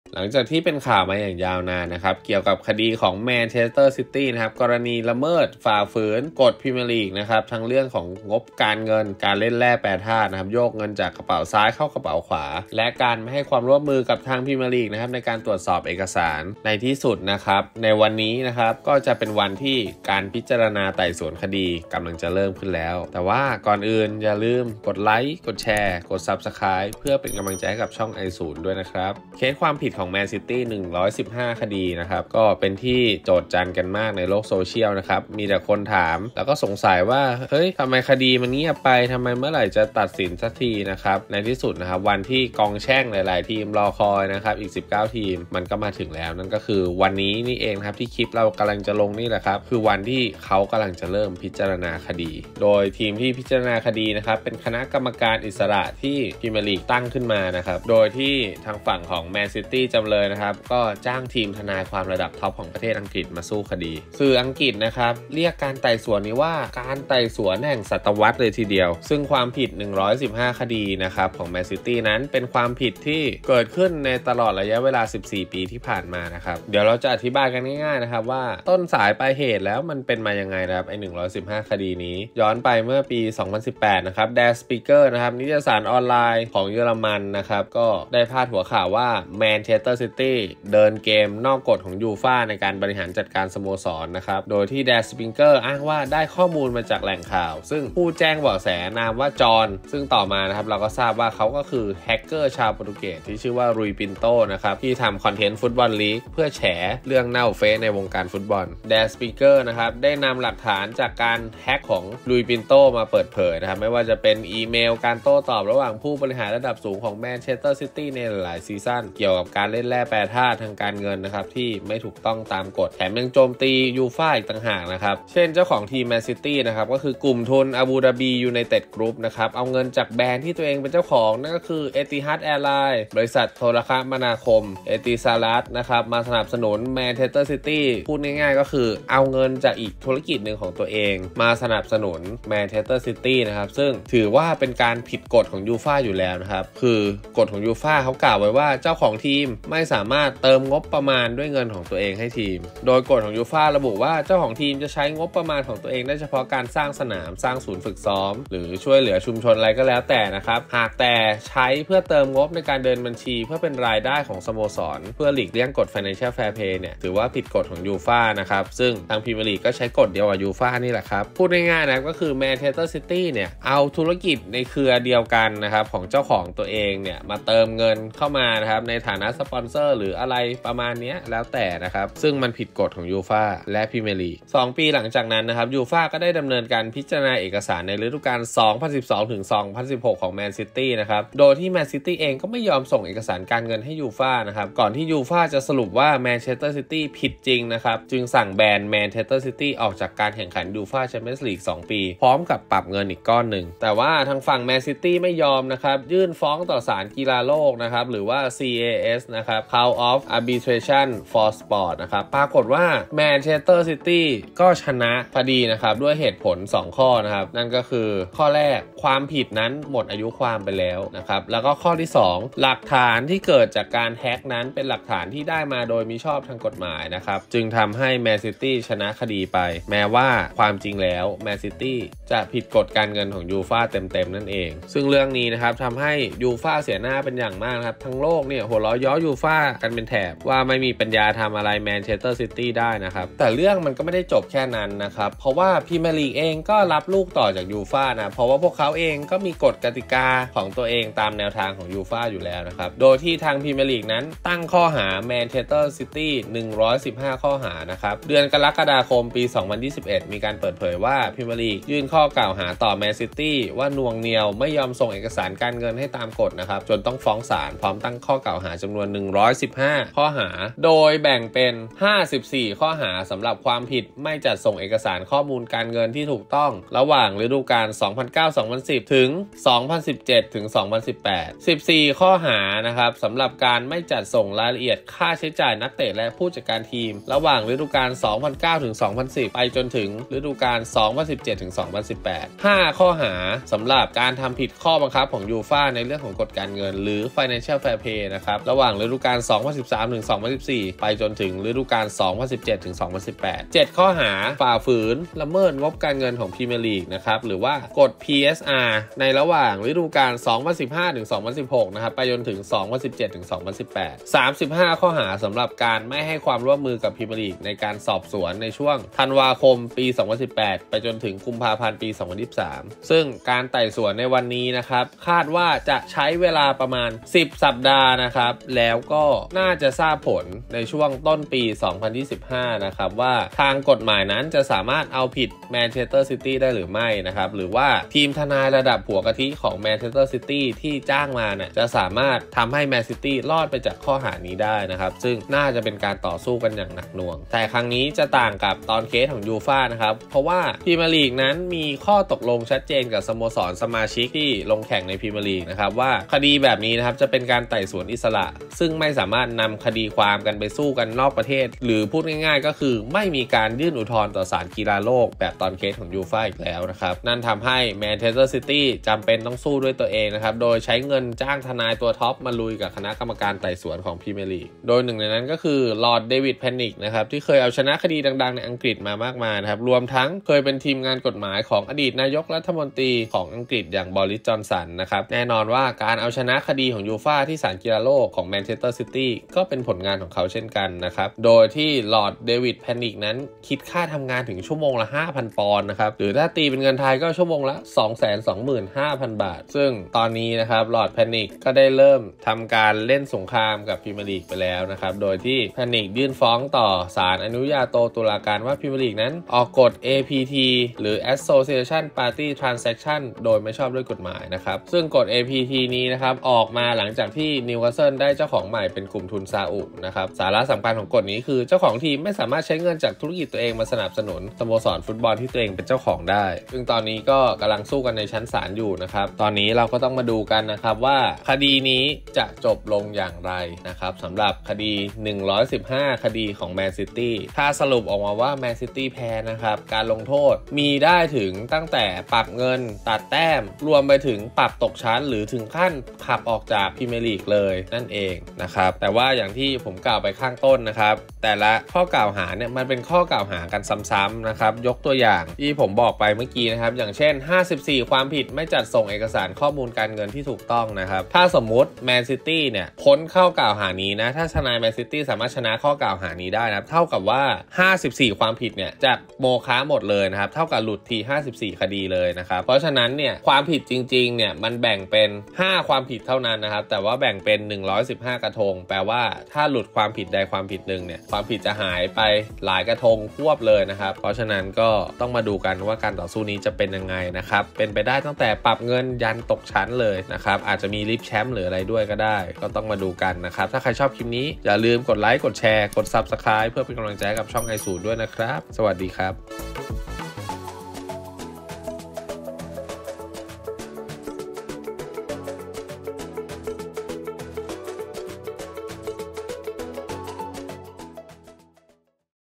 เริ่มขึ้นแล้ว 115 คดีของ แมนซิตี้ เริ่มไต่สวนวันนี้อาจรุนแรงสุดถึงขั้นปรับตกชั้น
ฝากติดตามเพจผมด้วยนะครับ